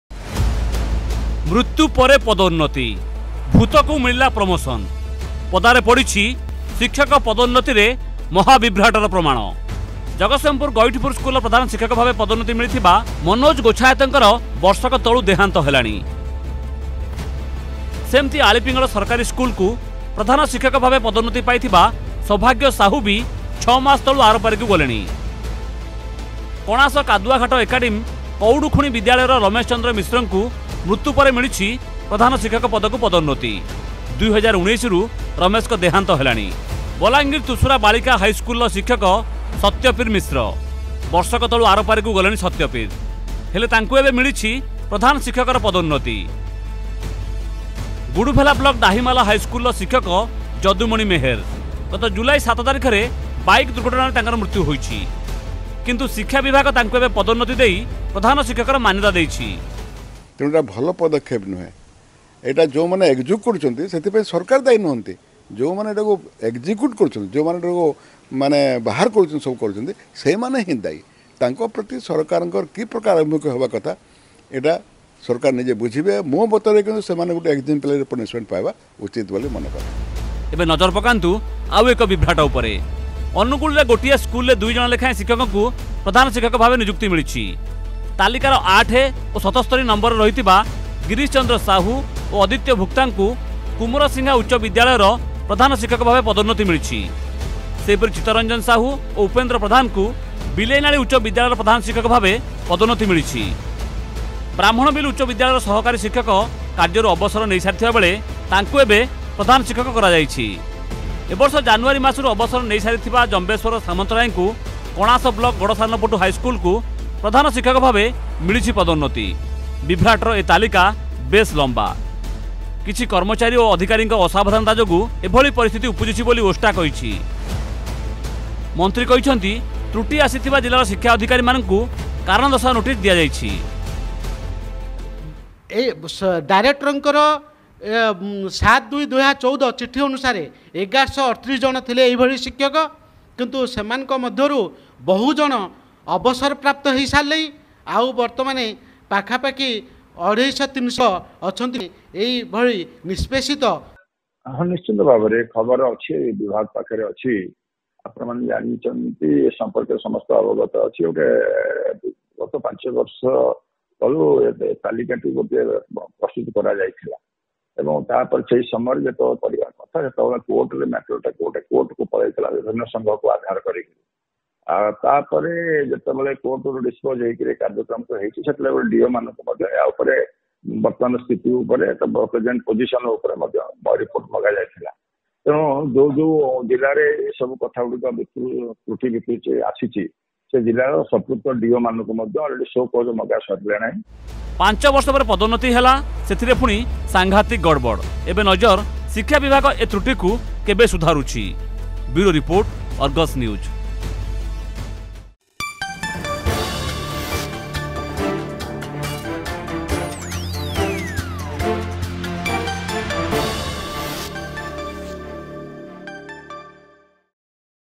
ଶିକ୍ଷା ବିଭାଗର ଅଜବ ନୀତି ।
ଶିକ୍ଷା ବିଭାଗର ଅଜବ ନୀତି । ପଦୋନ୍ନତି ପାଇବା ପାଇଁ ଶିକ୍ଷକ ବର୍ଷ ବର୍ଷ ଧରି ଅପେକ୍ଷା କଲେ ,ହେଲେ ତାଙ୍କୁ ଉଚିତ ସମୟରେ ପଦୋନ୍ନତି ମିଳିଲା ନାହିଁ ।  ତେବେ  ଯେତେବେଳେ ଇହଧାମ ତ୍ୟାଗ କରି ଶିକ୍ଷକ ଜଣକ ଆରପାରିକୁ ଚାଲିଗଲେ ସେତେବେଳେ ସରକାର ତାଙ୍କୁ ପଦୋନ୍ନତି ଦେଲେ । ଦେଖିବା ଅର୍ଗସର ସ୍ପେଶାଲ ରିପୋର୍ଟ ।